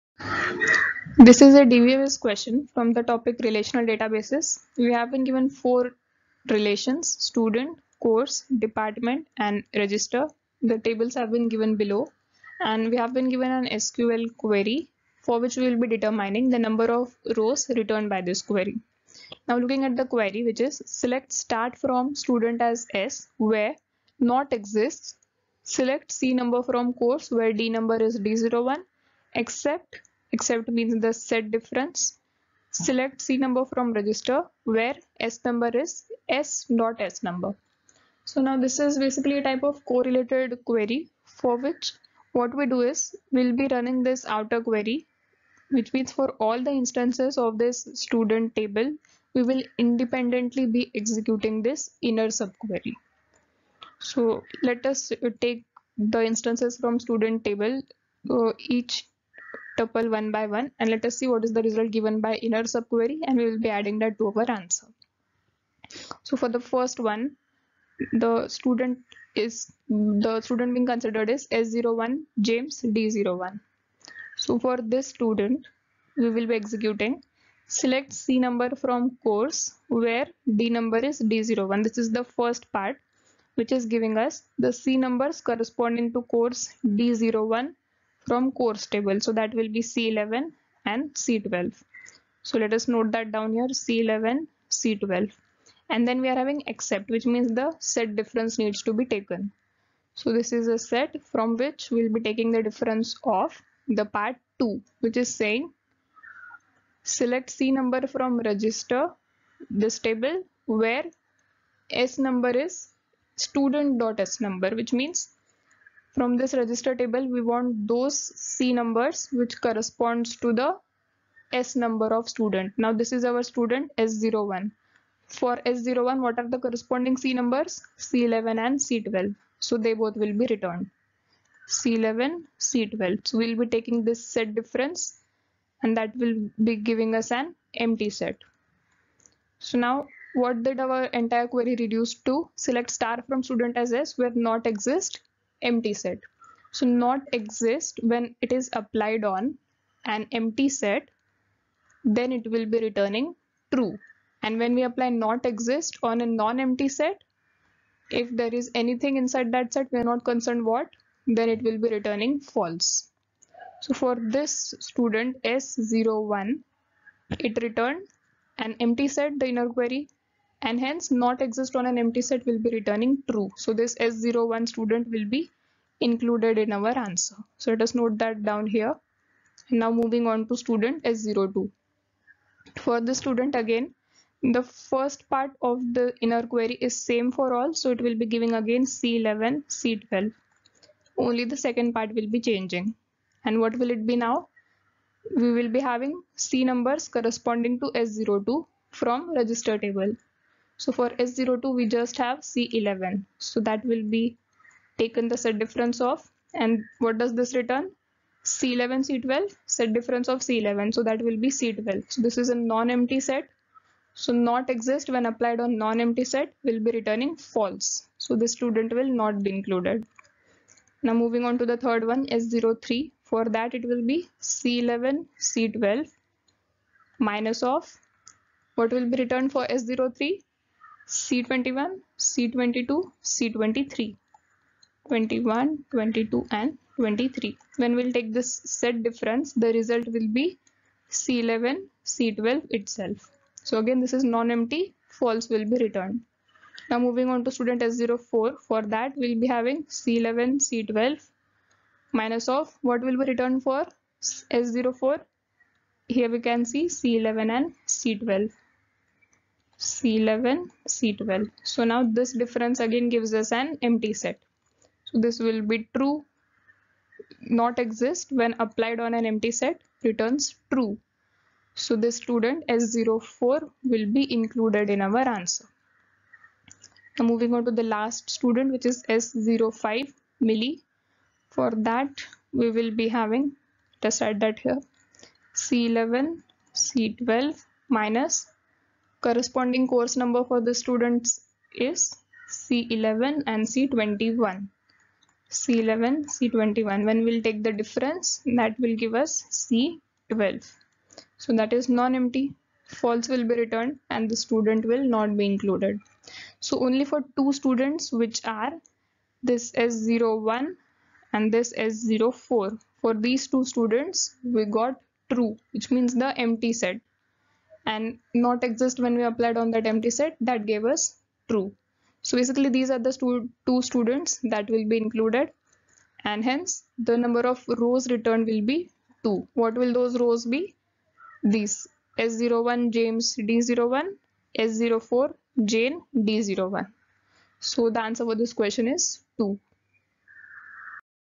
GATE 2022 | DBMS | SQL | GATE TEST SERIES | SOLUTIONS ADDA | EXPLAINED BY POOJA
GATE 2022 Q56: Consider the relational database with the following four schemas and their respective instances.
Student(sNo, sName, dNo) Dept(dNo, dName)
Course(cNo, cName, dNo)
Register(sNo, cNo)
The number of rows returned by the above SQL query is___________.